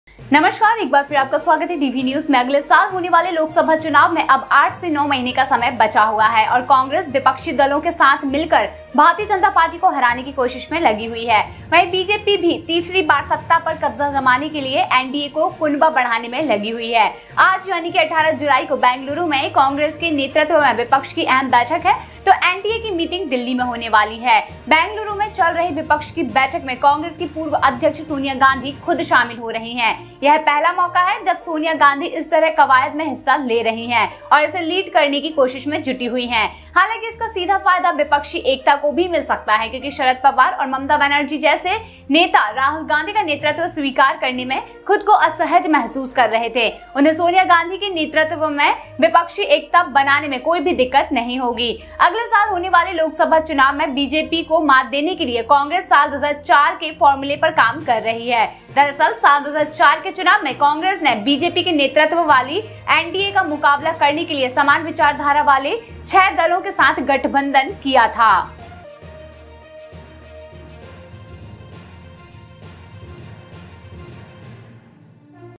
Congress Plan for 2024 Election: BJP को मात देने के लिए 2004 के फॉर्मूले पर काम कर रही Congress
अगले साल होने वाले लोकसभा चुनाव (Lok Sabha Election 2024) में अब 8-9 महीने का समय बचा है और कांग्रेस विपक्षी दलों के साथ मिलकर भारतीय जनता पार्टी (BJP) को हराने की कोशिश में लगी है. वहीं, बीजेपी भी तीसरी बार सत्ता पर कब्जा करने के लिए एनडीए को कुनबा बढ़ाने में लगी है. आज (18 जुलाई) बेंगलुरु में कांग्रेस के नेतृत्व में विपक्ष की अहम बैठक है, तो एनडीए की मीटिंग दिल्ली में होने वाली है.

There is now 8-9 months left in the next year's Lok Sabha Elections (Lok Sabha Election 2024) and the Congress, along with the opposition parties, is trying to defeat the Bharatiya Janata Party (BJP). At the same time, BJP is also engaged in increasing the NDA clan to capture power for the third time. Today (July 18) there is an important meeting of the opposition led by the Congress in Bengaluru, while the NDA meeting is going to be held in Delhi.